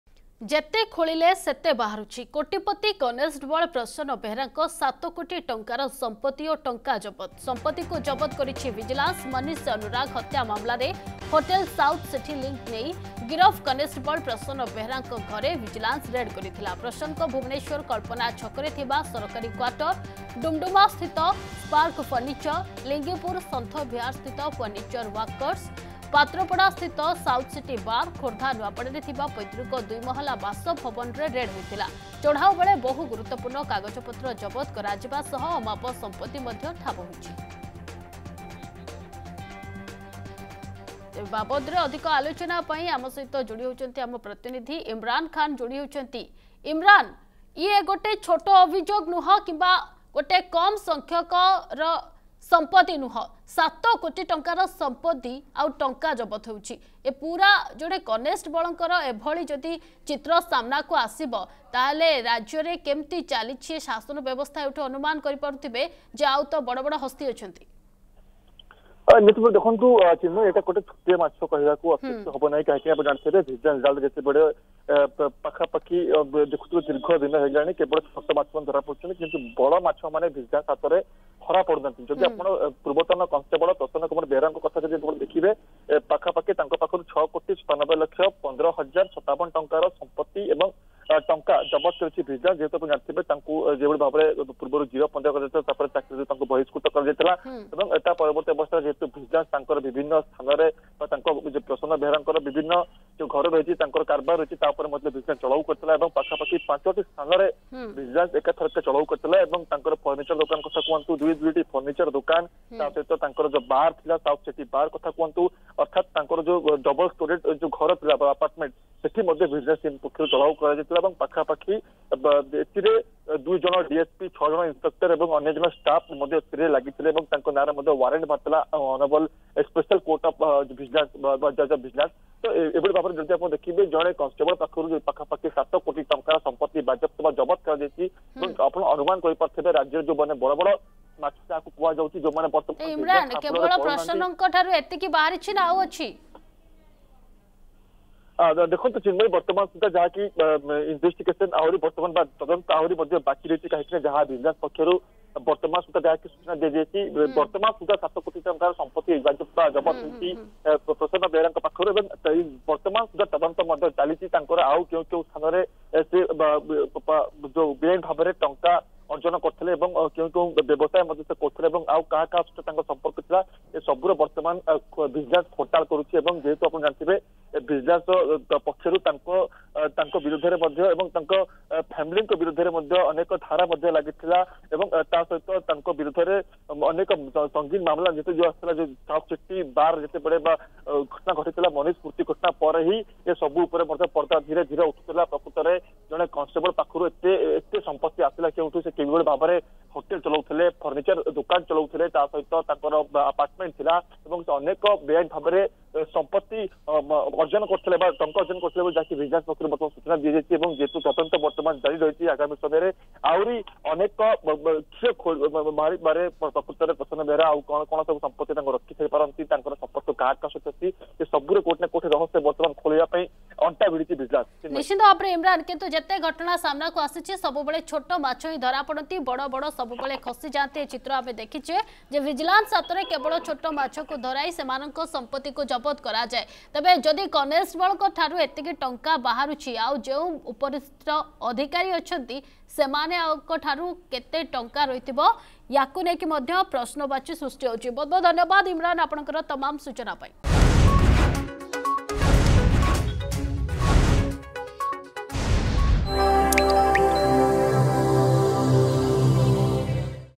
କୋଟିପତି କନଷ୍ଟେବଳଙ୍କ ପାଖରେ ଆଉ କେତେ?
ଯେତେ ଖୋଳିଲେ ସେତେ ବାହାରୁଛି । କୋଟିପତି କନଷ୍ଟେବଳ ପ୍ରସନ୍ନ ବେହେରାଙ୍କ 7 କୋଟି ଟଙ୍କାର ସମ୍ପତ୍ତି ଓ ଟଙ୍କା ଜବତ୍ । ମନୀଷ ଅନୁରାଗ ହତ୍ୟା ମାମଲାରେ ହୋଟେଲ ସାଉଥ ସିଟି ଲିଙ୍କ ମାମଲା ।